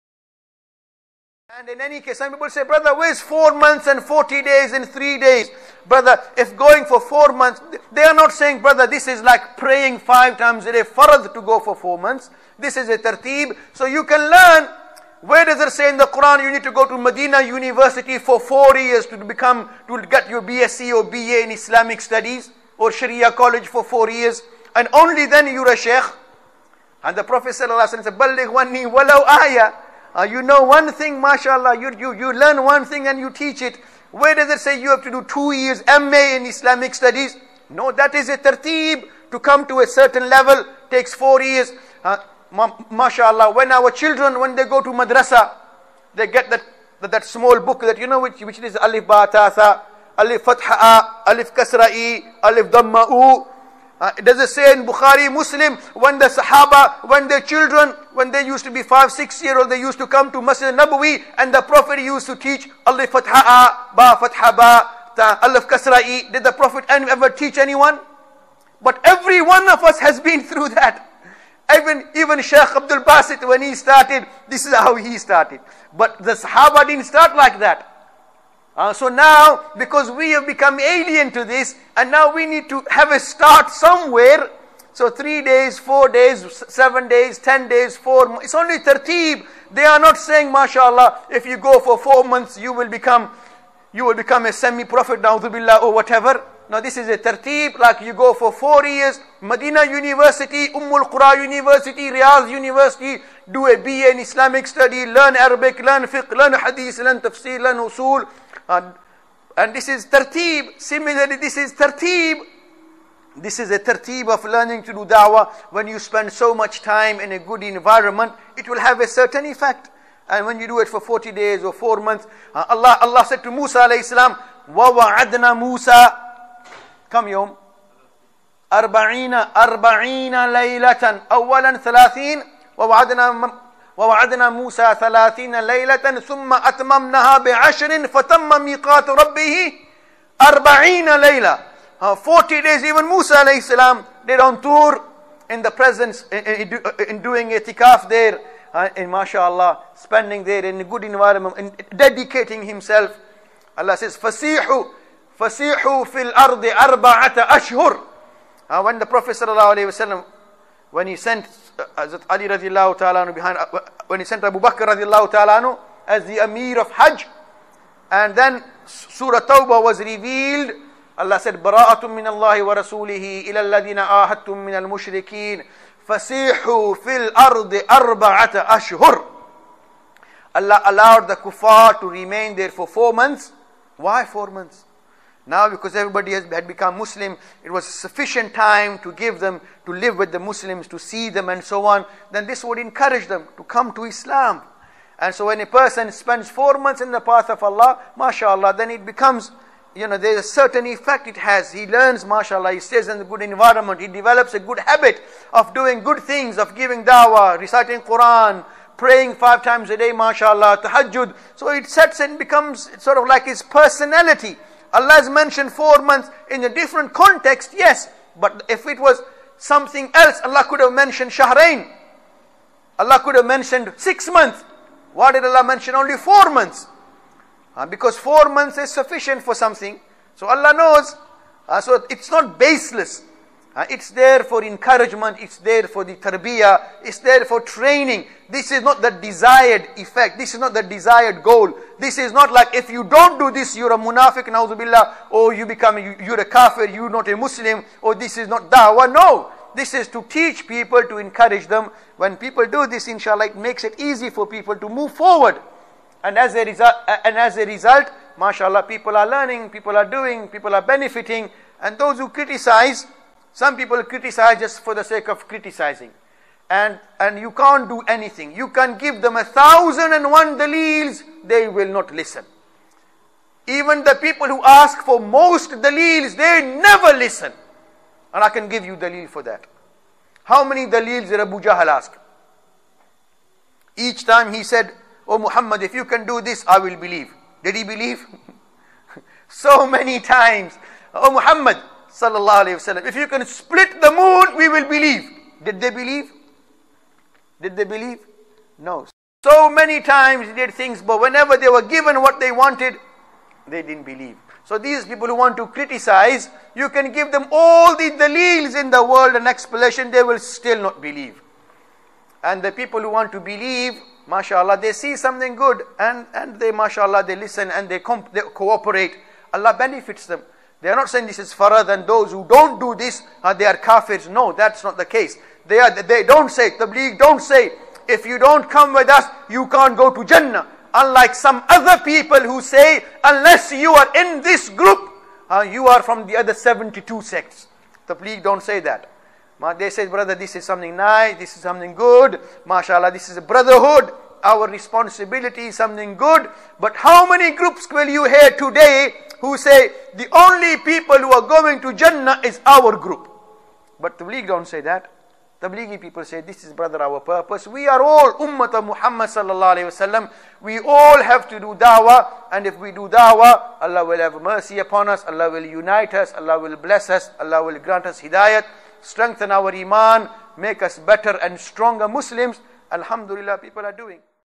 Where does 3 days, 40 days, 4 months come from? [Tablighi Jamaat]
english bayan about thableeq work- Ash Sheikh Mumtaz Ul Haq